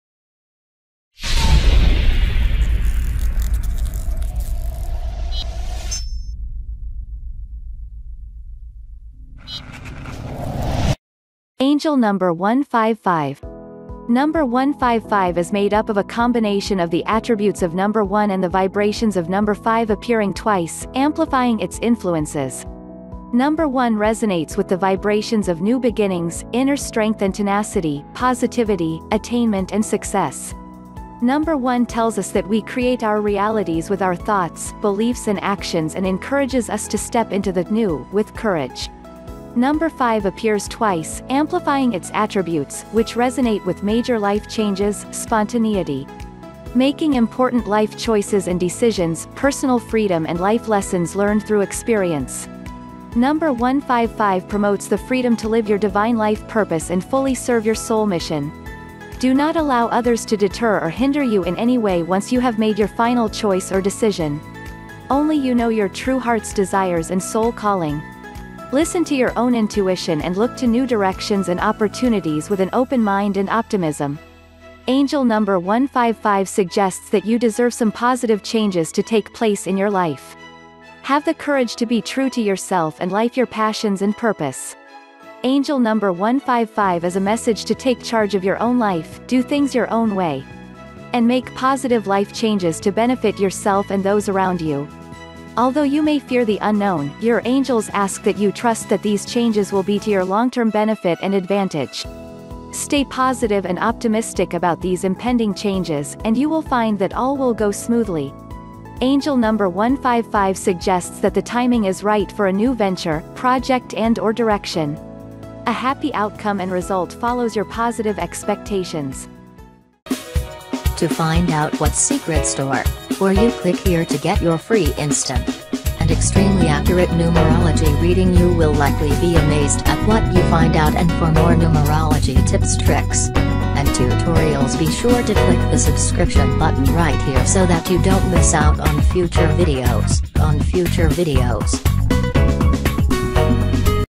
🔴 Angel Number Meanings 155 ✅ Seeing 155 ✅ Numerology Box
Number 155_meaning is made up of a combination of the attributes of number 1 and the vibrations of number 5 appearing twice, amplifying its influences. Number 1 resonates with the vibrations of new beginnings, inner-strength and tenacity, positivity, attainment and success. Number 1 tells us that we create our realities with our thoughts, beliefs and actions and encourages us to step into the ‘new’ with courage. Number 5 appears twice, amplifying its attributes, which resonate with major life changes, spontaneity, making important life choices and decisions, personal freedom and life lessons learned through experience. Number 155 promotes the freedom to live your Divine life purpose and fully serve your soul mission.

Do not allow others to deter or hinder you in any way once you have made your final choice or decision. Only you know your true heart’s desires and soul calling. Listen to your own intuition and look to new directions and opportunities with an open mind and optimism.

angel_number_155 suggests that you deserve some positive changes to take place in your life. Have the courage to be true to yourself and life your passions and purpose.

Angel Number 155 is a message to take charge of your own life, do things your own way, and make positive life changes to benefit yourself and those around you. Although you may fear the unknown, your angels ask that you trust that these changes will be to your long-term benefit and advantage. Stay positive and optimistic about these impending changes, and you will find that all will go smoothly.

Angel Number 155 suggests that the timing is right for a new venture, project and/or direction. A happy outcome and result follows your positive expectations.